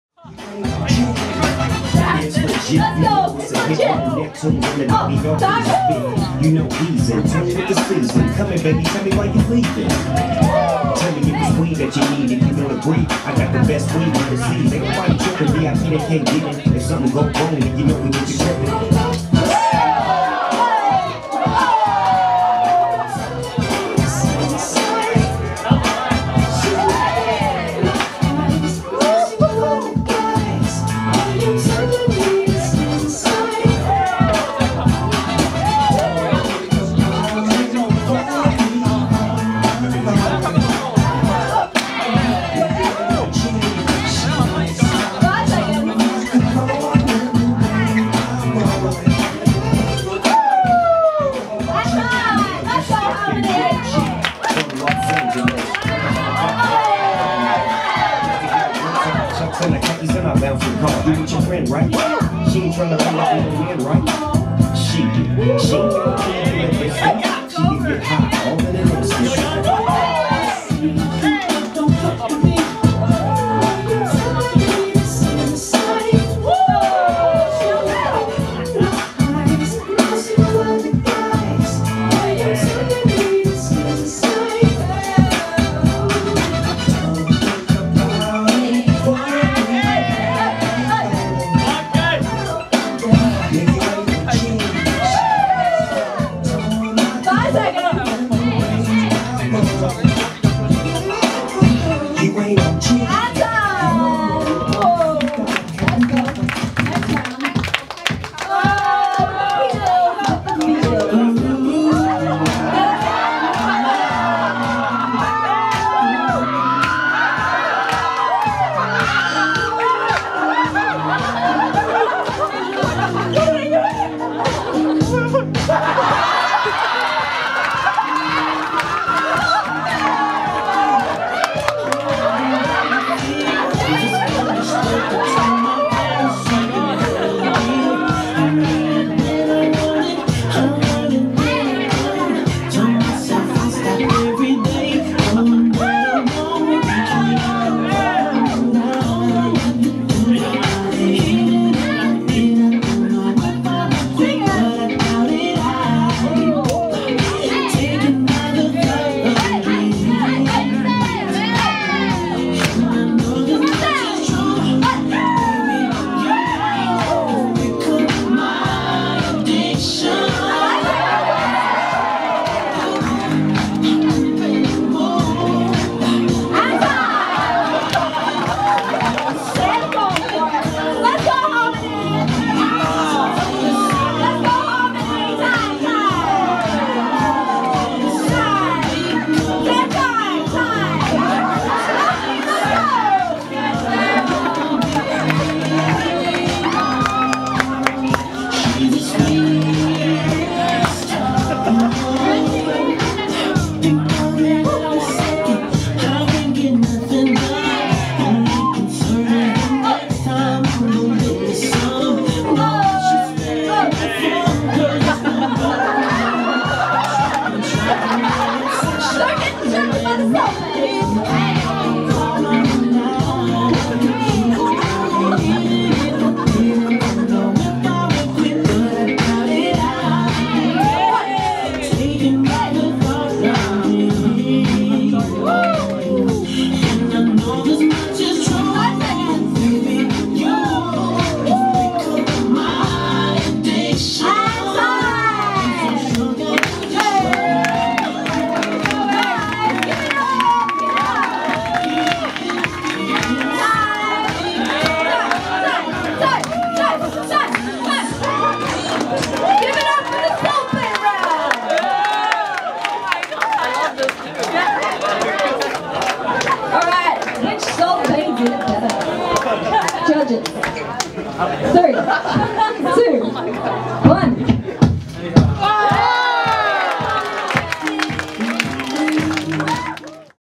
Musical Adversity RnB Battle Halmony vs Ty Rex Top 16
Top 16:  Halmony vs Ty Rex
Event: Musical Adversity 2v2 All Styles Rnb Battle.
Location: Crossover Dance Studios
Date: 11/02/2017